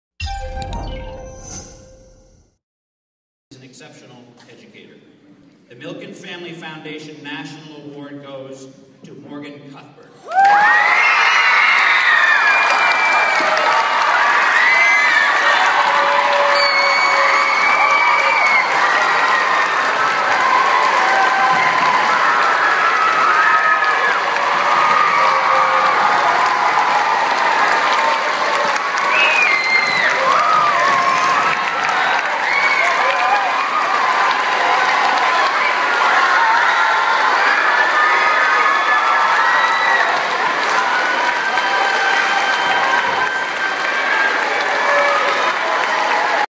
Maine Teacher Wins Milkin Award
A teacher at Frank H. Harrison Middle School in Yarmouth received a prestigious award Monday afternoon.